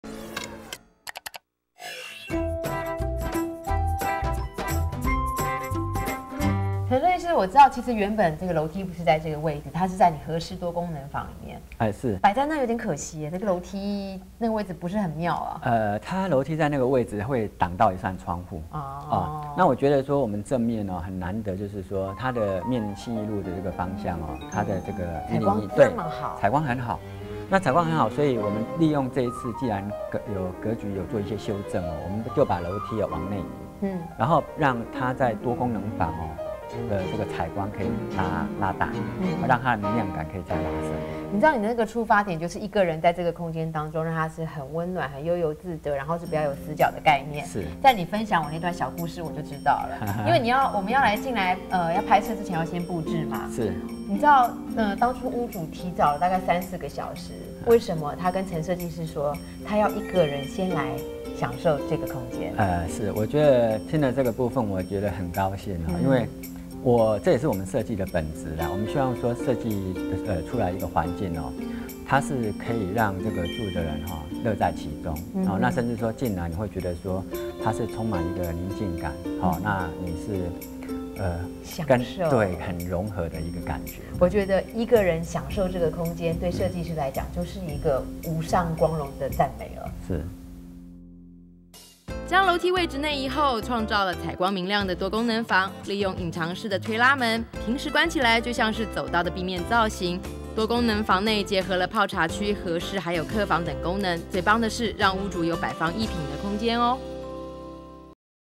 簡約時尚深呼吸 品味豪宅之最《樓梯／多功能房》【富億設計-陳錦樹】
詳情請至幸福空間網站位移至餐廳處的樓梯改以光梯懸空設計，彷似躍動向上的音符，更是串連兩個藝文場域的裝置藝術作品。